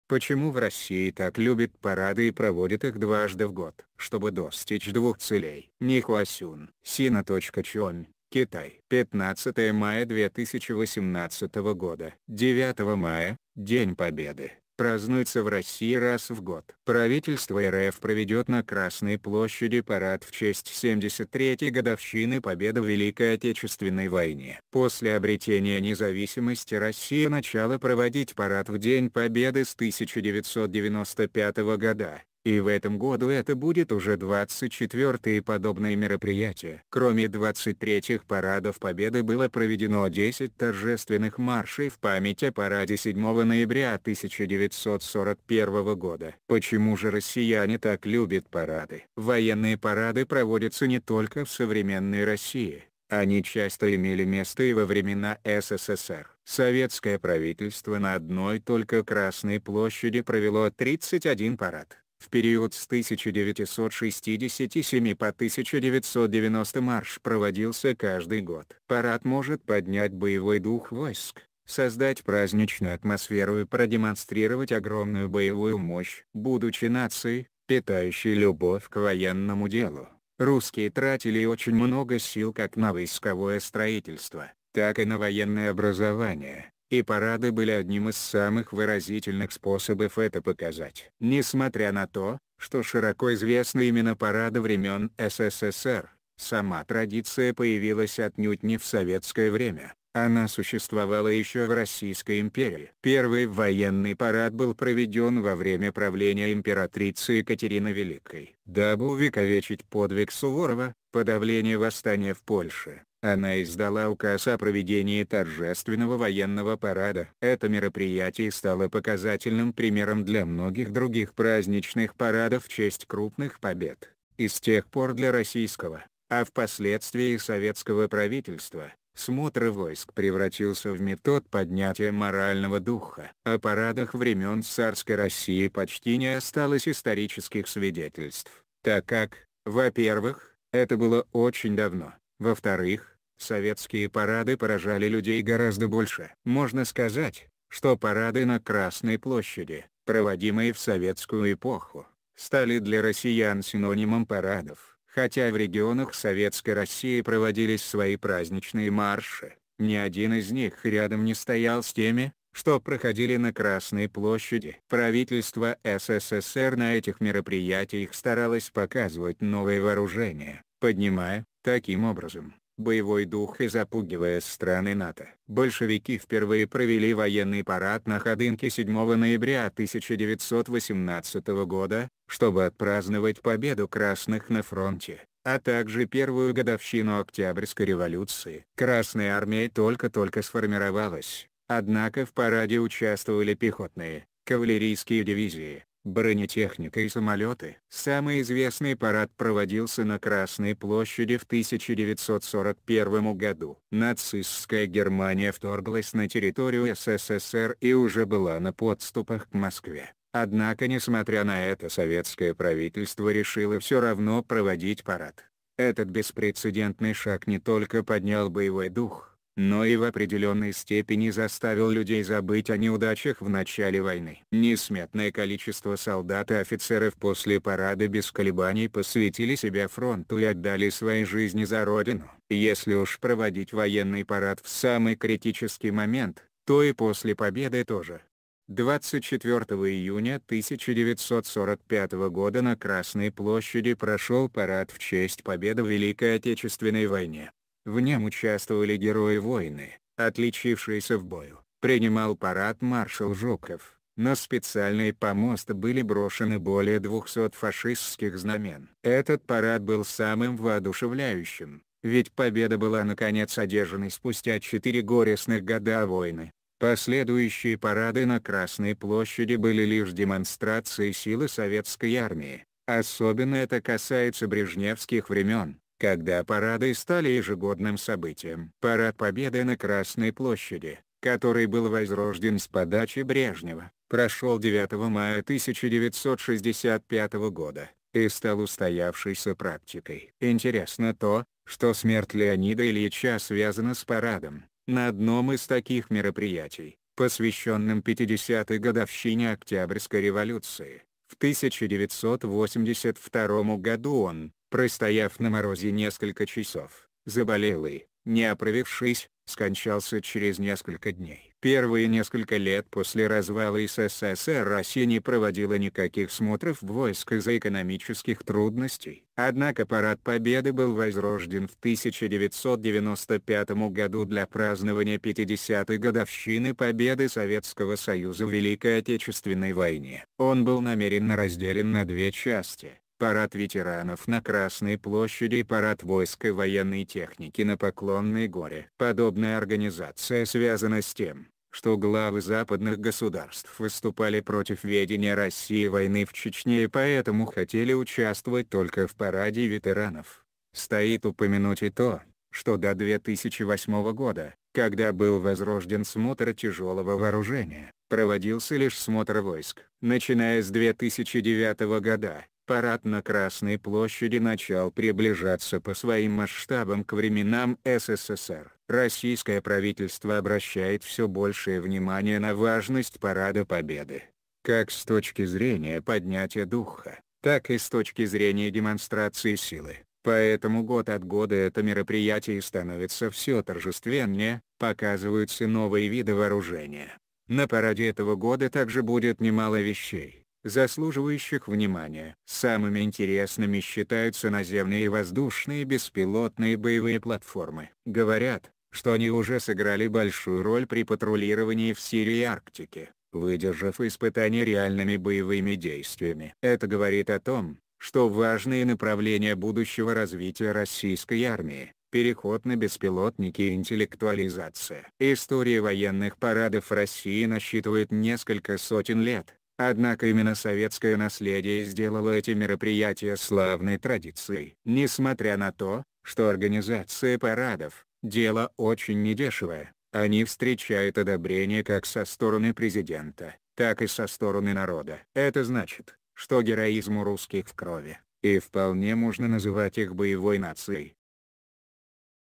Почему в России так любят парады и проводят их дважды в год? Чтобы достичь двух целей.
Sina.com, Китай.
Не Хуасюн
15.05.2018

9 мая — День победы, празднуется в России раз в год. Правительство РФ проведет на Красной площади парад в честь 73-й годовщины победы в Великой Отечественной войне. После обретения независимости Россия начала проводить парад в День Победы с 1995 года, и в этом году это будет уже 24-е подобное мероприятие. Кроме 23-х парадов победы было проведено десять торжественных маршей в память о параде 7 ноября 1941 года. Почему же россияне так любят парады?    Военные парады проводятся не только в современной России, они часто имели место и во времена СССР. Советское правительство на одной только Красной площади провело 31 парад, в период с 1967 по 1990 марш проводился каждый год. Парад может поднять боевой дух войск, создать праздничную атмосферу и продемонстрировать огромную боевую мощь. Будучи нацией, питающей любовь к военному делу, русские тратили очень много сил как на войсковое строительство, так и на военное образование, и парады были одним из самых выразительных способов это показать.   Несмотря на то, что широко известны именно парады времен СССР, сама традиция появилась отнюдь не в советское время, она существовала еще в Российской империи. Первый военный парад был проведен во время правления императрицы Екатерины Великой. Дабы увековечить подвиг Суворова (подавление восстания в Польше), она издала указ о проведении торжественного военного парада. Это мероприятие стало показательным примером для многих других праздничных парадов в честь крупных побед, и с тех пор для российск...